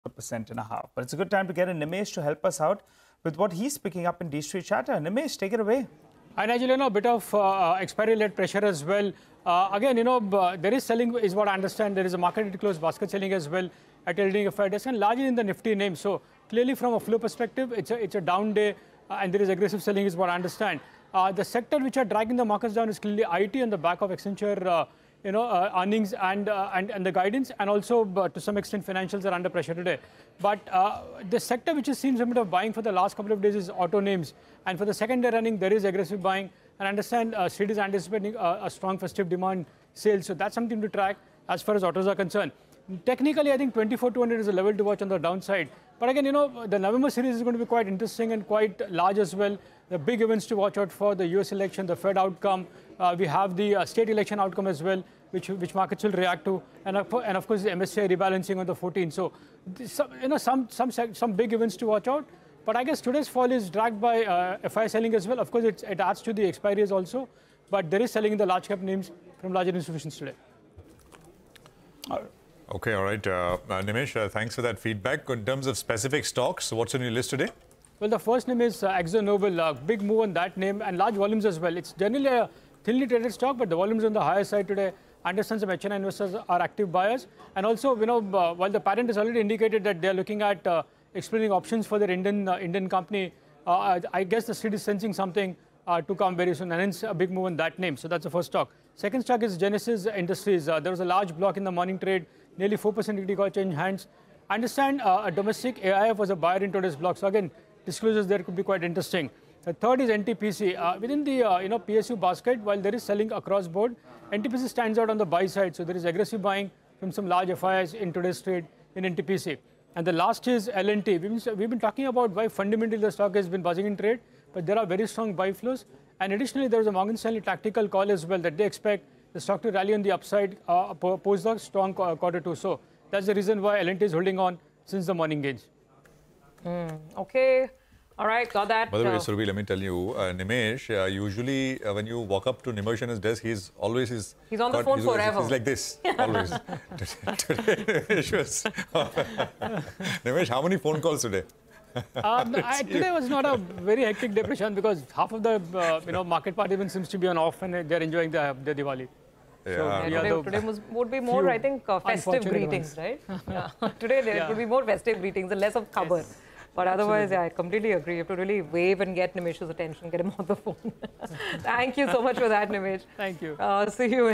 D-Street Chatter: What's Buzzing At The Dealers' Desk? | CNBC TV18
D-Street Chatter | Market under pressure dragged by I.T. & financials stocks. Nifty support around 24,200 crucial. Akzo Nobel, Genesys, NTPC & L&T buzzing in trade. Dealing room chatter with Nimesh Shah.

n18oc_business